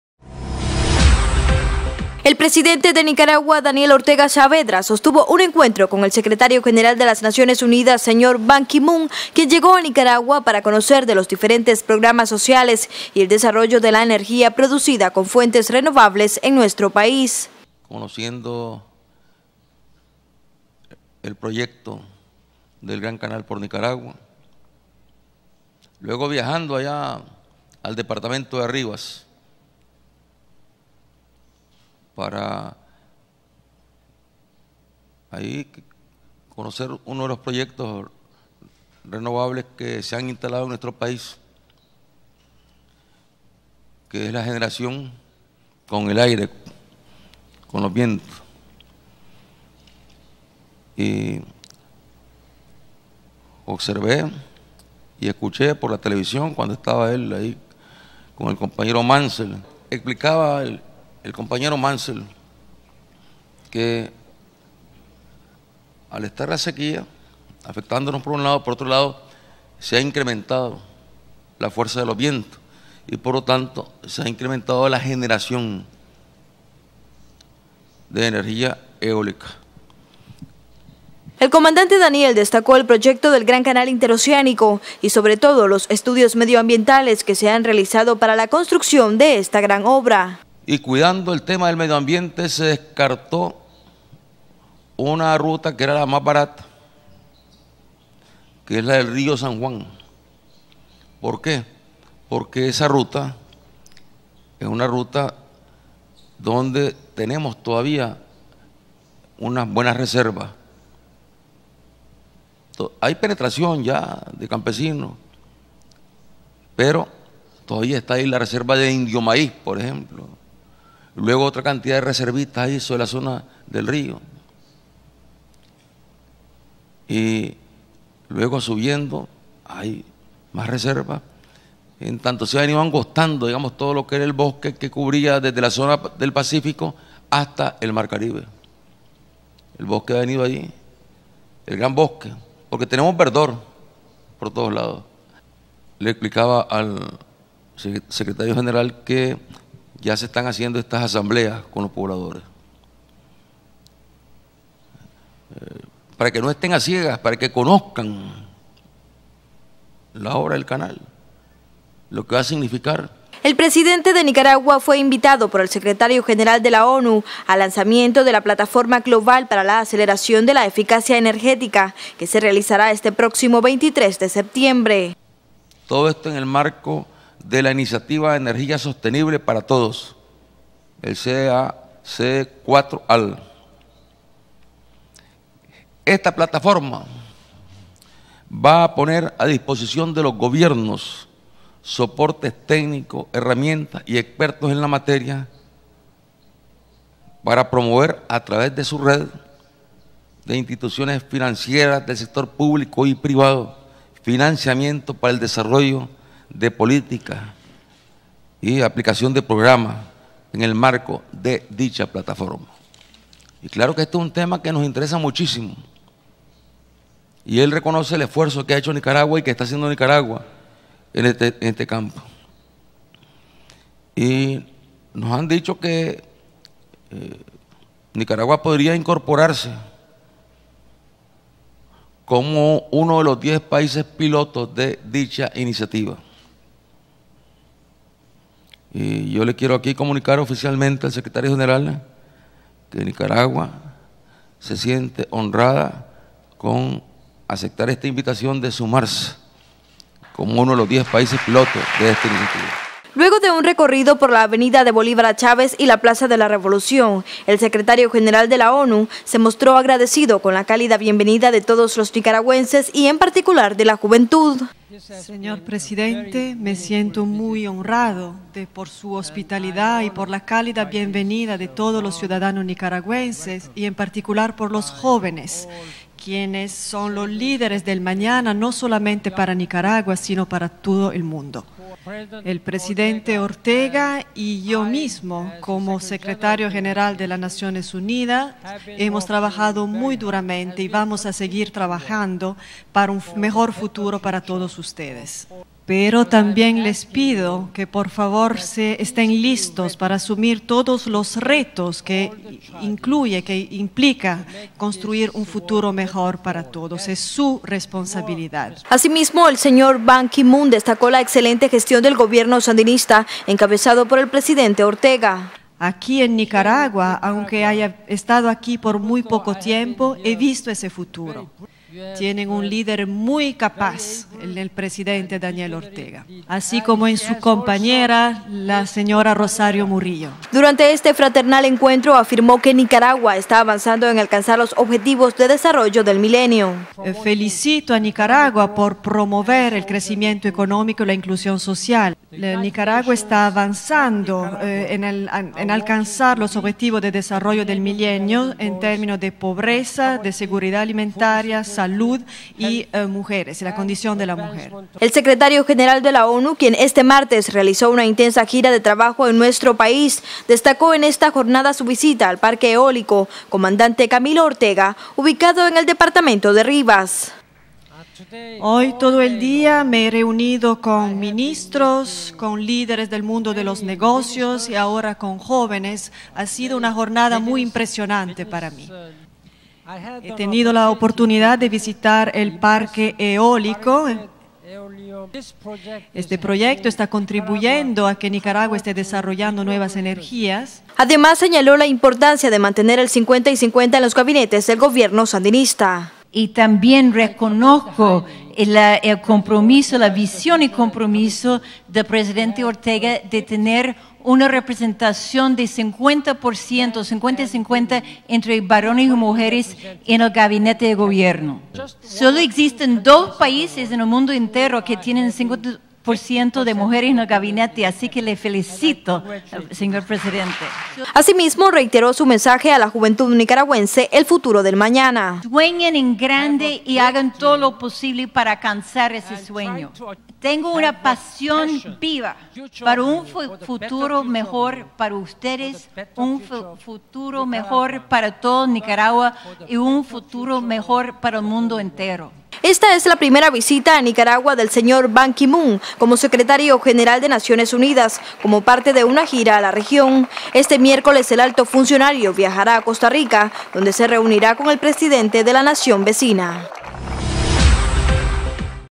Secretario General de la ONU sostuvo fraternal encuentro con el Comandante Daniel
Primer noticiero virtual de Nicaragua.